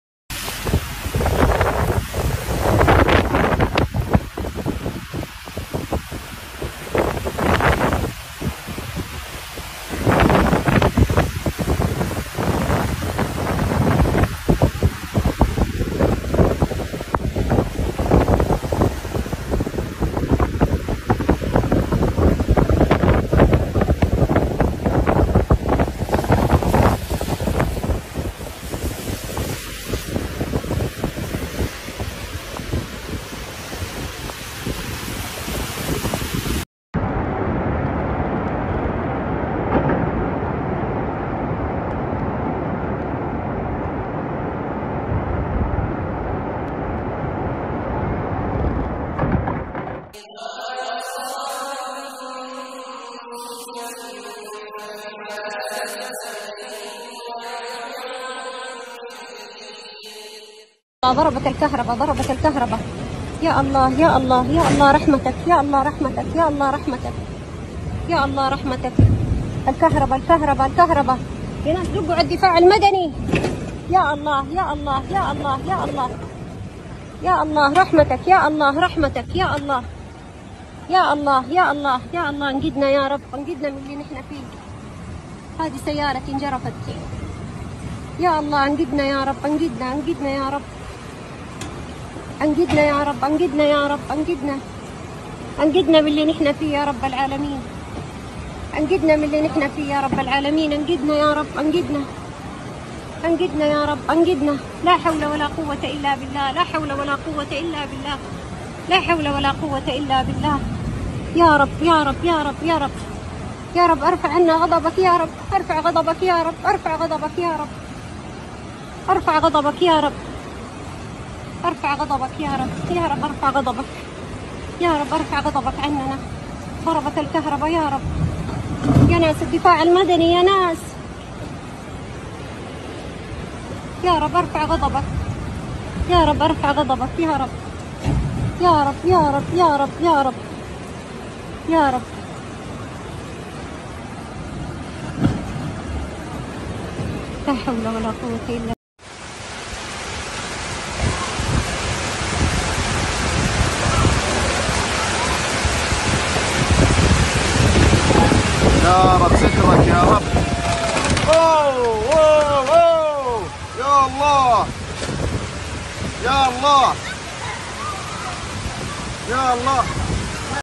INNALILLAHI🔥ARAB SAUDI,MOBIL2 TENGGELAM#elmahdy official#umihaura tv#jeddah#mekkah
ASTAGFIRULLAH🔥JEDDAH BANJIR BESAR MOBIL2 HANYUT,INNALILLAHI@elmahdyaceh @umihauratv